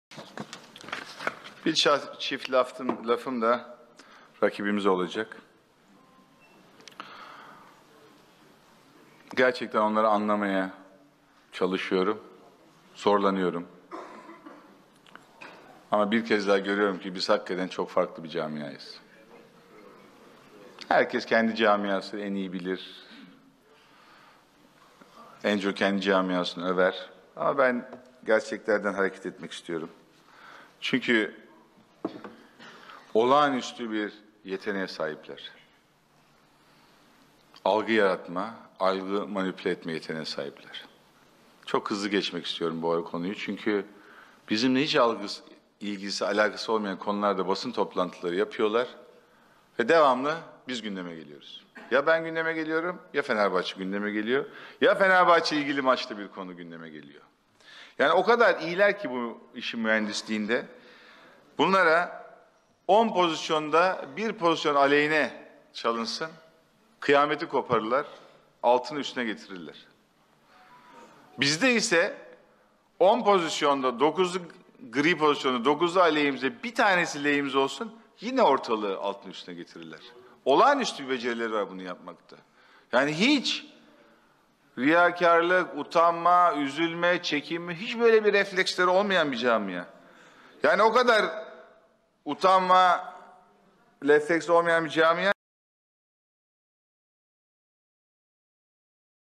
Ali Koç: “Rakibimizi gerçekten anlamaya çalışıyorum. Zorlanıyorum.
Ali Koç: “Rakibimizi gerçekten anlamaya çalışıyorum. Zorlanıyorum. Rakibimiz; hiç riyakarlık, utanma, üzülme, çekinme refleksi olmayan bir camia."